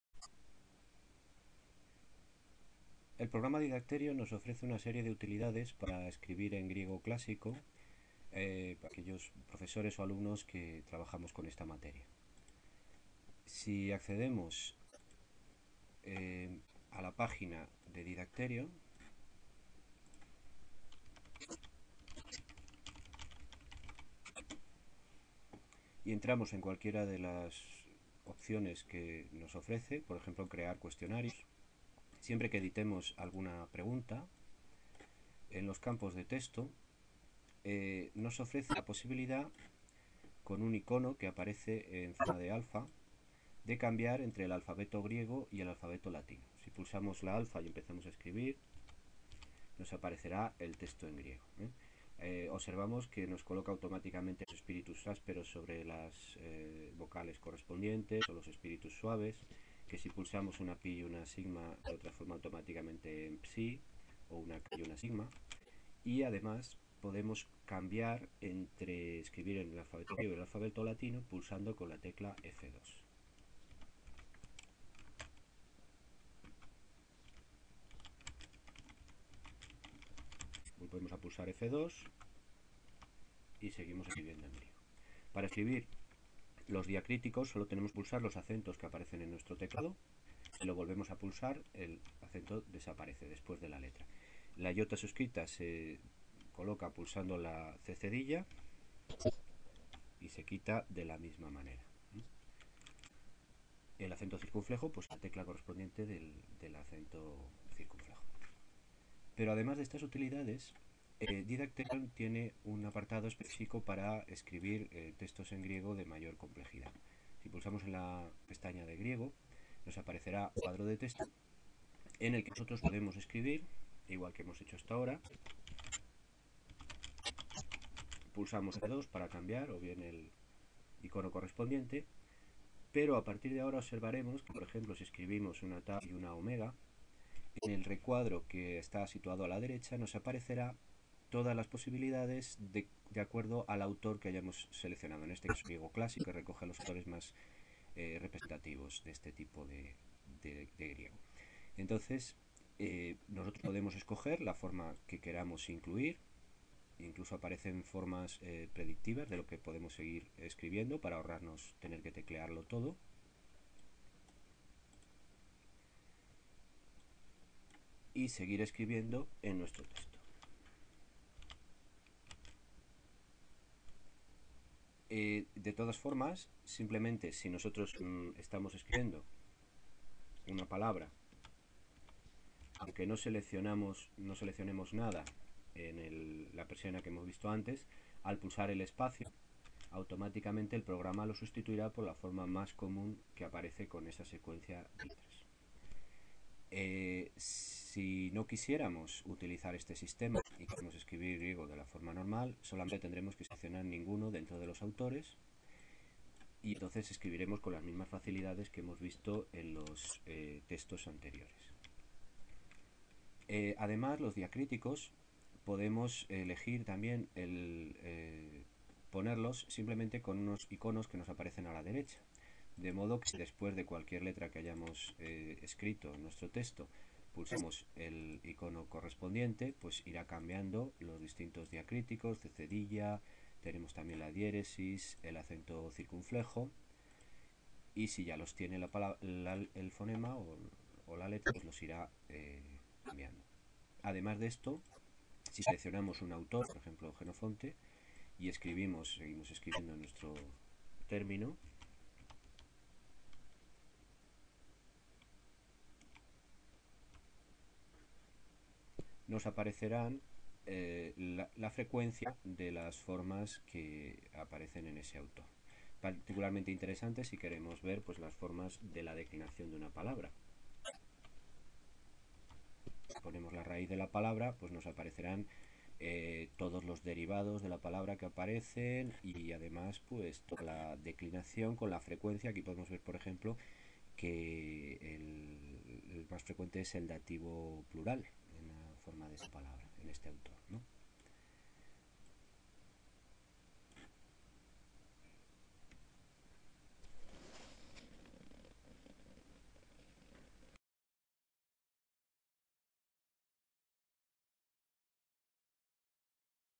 Utilidades para escribir en griego con didacTerion
Un conjunto de utilidades que nos permiten escribir griego clásico en línea de forma cómoda con una base de datos que nos ofrece los términos utilizados por los distintos autores de la Grecia Clásica. Además incluye automáticamente los espíritus iniciales correspondientes y permite alternar en el mismo cuadro de texto entre alfabeto latino y alfabeto griego pulsando la tecla F2. Utiliza un método similar a Sibylla.